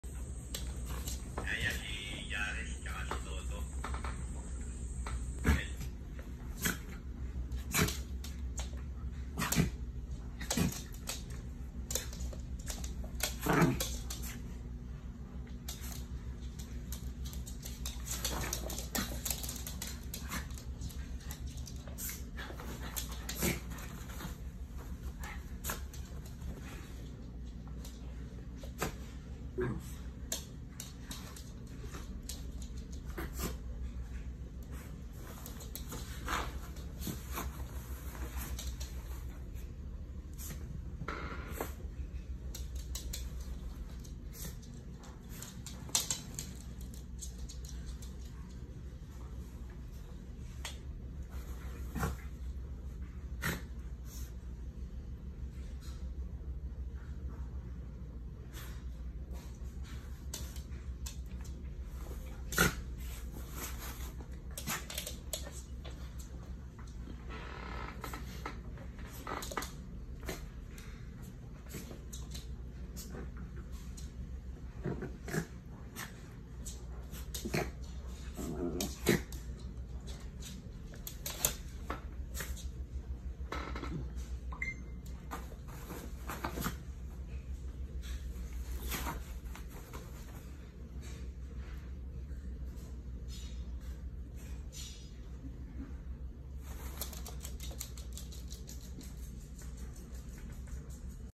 Lilo and Stitch fighting over the bed and blanket.
They got a new bed and fighting over it.  Stitch win.  I should have bought bigger one.